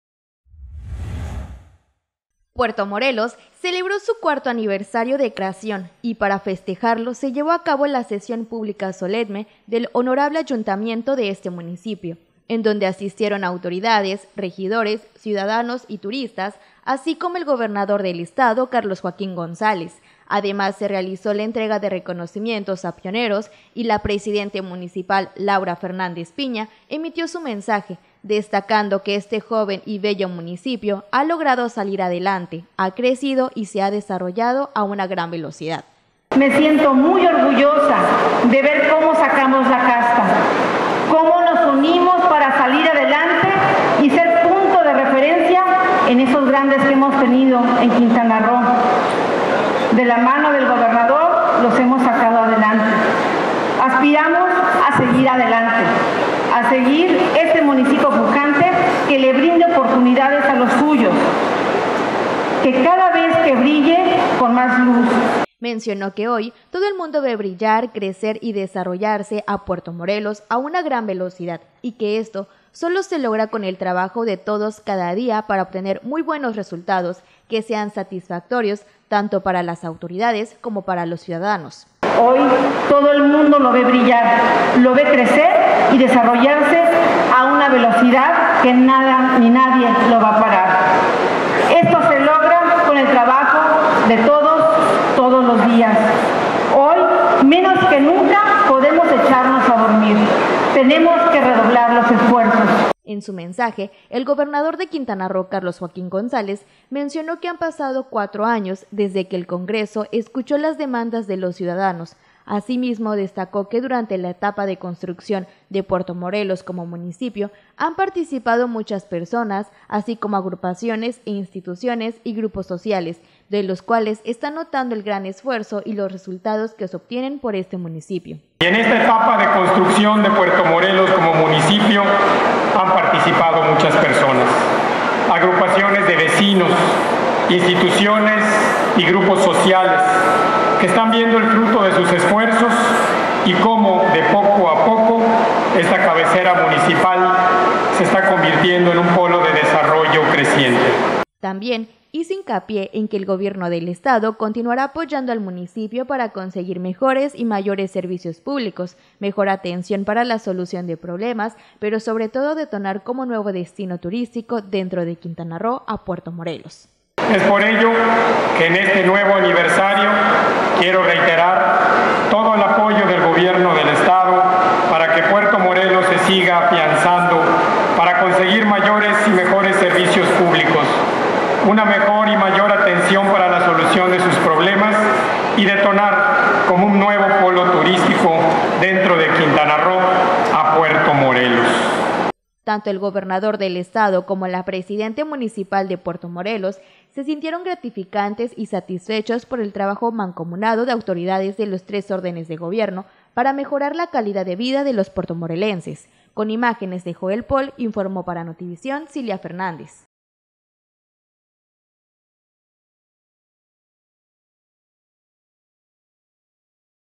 Sesión pública solemne en Puerto Morelos
Para festejar el cuarto aniversario de Puerto Morelos se realizó la Sesión Pública Solemne del Honorable Ayuntamiento de este municipio, en donde estuvo presente el Gobernador de Quintana Roo, Carlos Joaquín González, la Presidente Municipal Laura Fernández y autoridades.